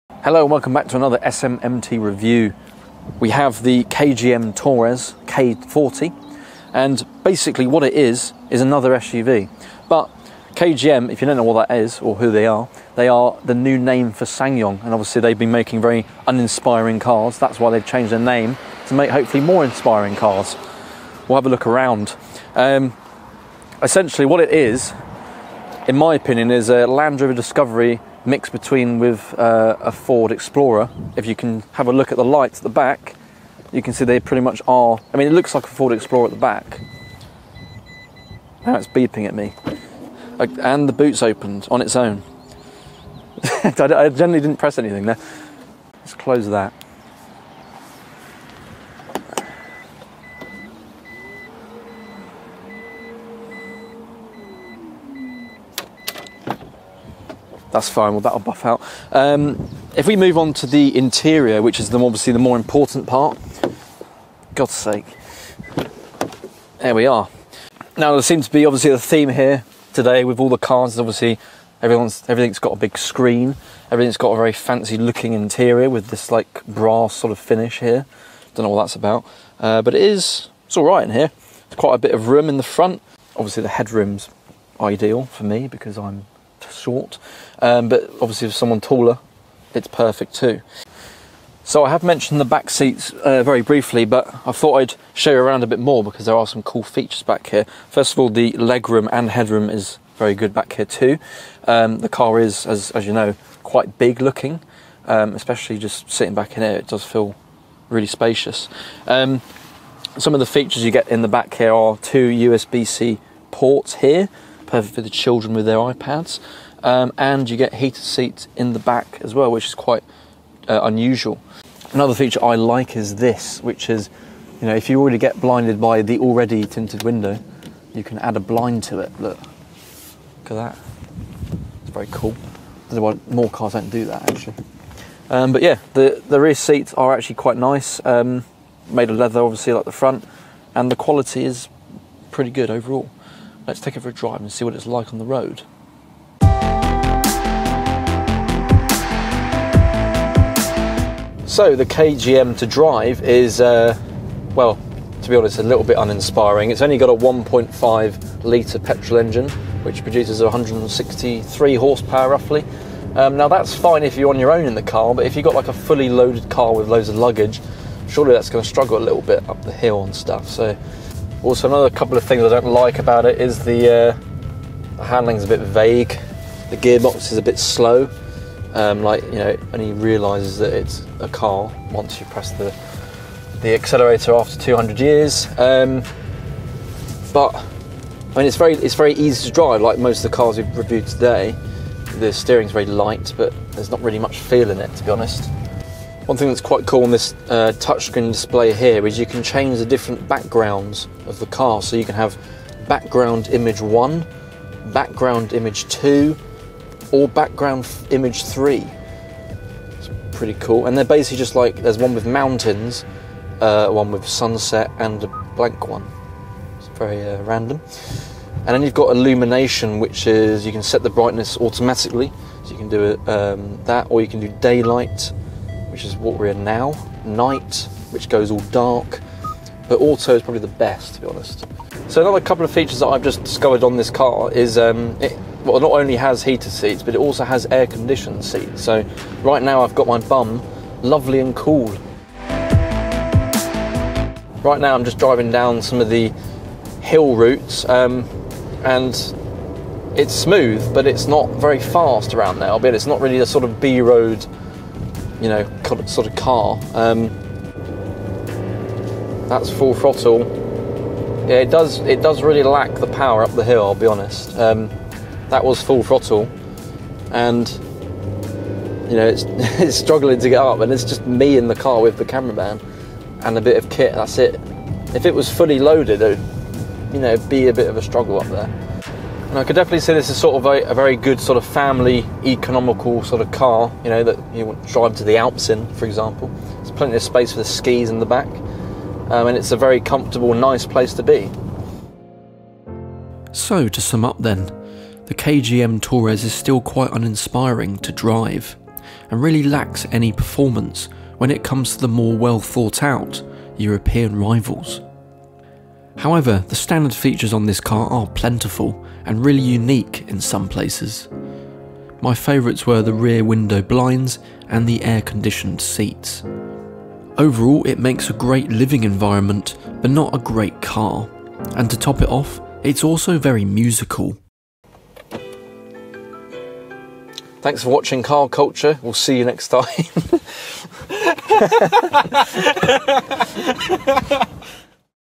NEW KGM Motors Torres K40 - Is it a Good SUV?
KGM, also formally known as Ssangyong, has made this new SUV called the Torres. Looks like a Ford Explorer, but is it as good as one? In this review we see if the Torres K40 is a good SUV.

This review was part of the SMMT Test Day we attended a few weeks ago. We know that this review isn't as in depth or as long as our other ones are and that's because we had limited time with the car! Either way, we hope you enjoyed it.

Follow us on Tik-Tok - @carcultureclubyt